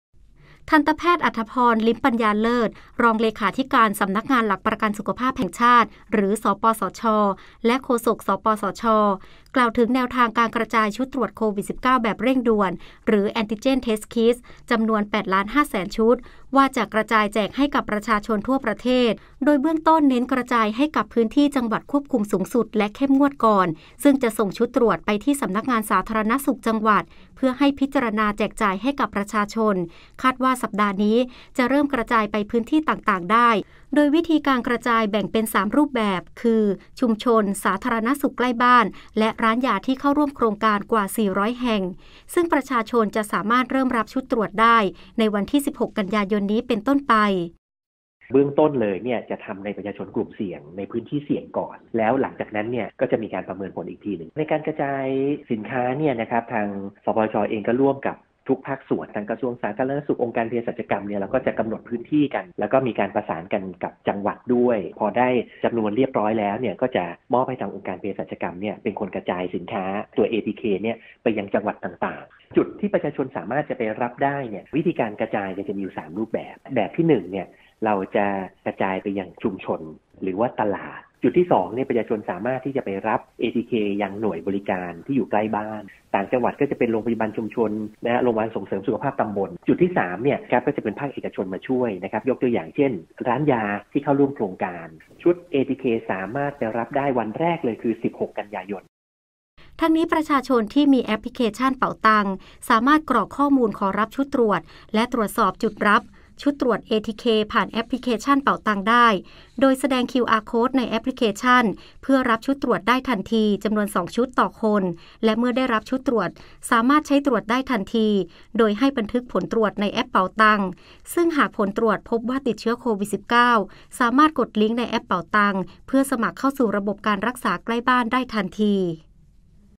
สปสช. เตรียมแจกจ่ายชุดตรวจโควิด-19 แบบเร่งด่วน (ATK) แก่ประชาชนทั่วประเทศ 8.5 ล้านชุด
สำนักงานหลักประกันสุขภาพแห่งชาติ เตรียมแจกจ่ายชุดตรวจโควิด-19 แบบเร่งด่วน (ATK) แก่ประชาชนทั่วประเทศ 8,500,000 ชุด ในวันที่ 16 กันยายนนี้

ทันตแพทย์ อรรถพร ลิ้มปัญญาเลิศ รองเลขาธิการสำนักงานหลักประกันสุขภาพแห่งชาติ (สปสช.) และ โฆษก สปสช. กล่าวถึงแนวทางการกระจายชุดตรวจโควิด-19 แบบเร่งด่วน ( Antigen test kit (ATK) จำนวน 8,500,000 ชุด ว่า จะกระจายแจกให้กับประชาชนทั่วประเทศ โดยเบื้องต้นเน้นกระจายให้กับพื้นที่จังหวัดควบคุมสูงสุดและเข้มงวดก่อน ซึ่งจะส่งชุดตรวจไปที่สำนักงานสาธารณสุขจังหวัดเพื่อให้พิจารณาแจกจ่ายให้กับประชาชน คาดว่าสัปดาห์นี้จะเริ่มกระจายไปพื้นที่ต่าง ๆ ได้ โดยวิธีการกระจายแบ่งเป็น 3 รูปแบบ คือ ชุมชน , สาธารณสุขใกล้บ้าน และร้านยาที่เข้าร่วมโครงการกว่า 400 แห่ง ซึ่งประชาชนจะสามารถเริ่มรับชุดตรวจได้ในวันที่ 16 กันยายนนี้ เป็นต้นไป

ทั้งนี้ ประชาชนที่มีแอปพลิเคชันเป๋าตัง สามารถกรอกข้อมูลขอรับชุดตรวจและตรวจสอบจุดรับชุดตรวจ ATK ผ่านแอปเป๋าตังได้ โดยแสดงคิวอาร์โคดในแอปพลิเคชั่นเพื่อรับชุดตรวจได้ทันที จำนวน 2 ชุดต่อคน และเมื่อได้รับชุดตรวจสามารถใช้ตรวจได้ทันที โดยให้บันทึกผลตรวจในแอปเป๋าตัง ซึ่งหากผลตรวจพบว่าติดเชื้อโควิด -19 สามารถกดลิงค์ในแอปเป๋าตังเพื่อสมัครเข้าสู่ระบบการรักษาใกล้บ้านได้ทันที